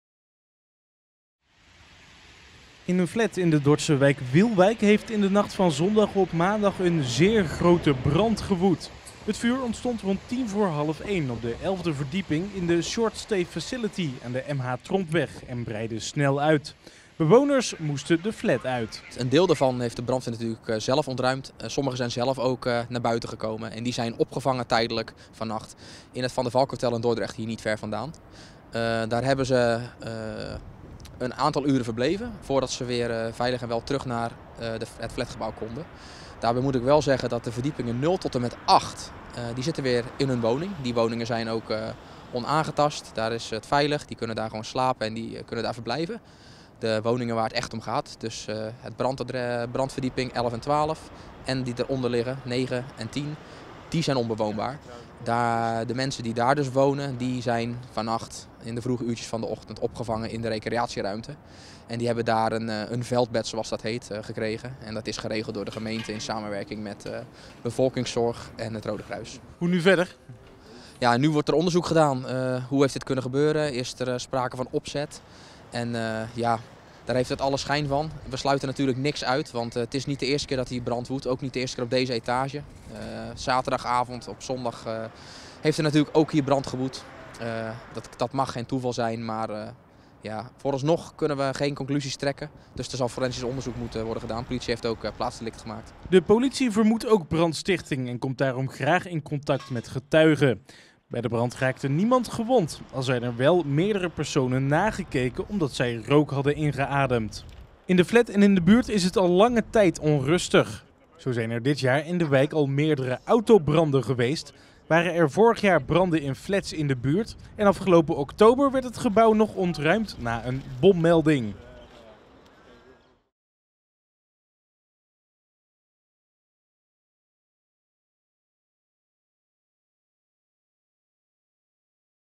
Grote brand op bovenste etages van flat in Dordrecht, schade is enorm
In een flat in de Dordtse wijk Wielwijk heeft in de nacht van zondag op maandag een zeer grote brand gewoed. Het vuur ontstond rond tien voor half één op de elfde verdieping in de Short Stay Facility aan de MH Trompweg en breidde snel uit. Bewoners moesten de flat uit.

Meer lezen?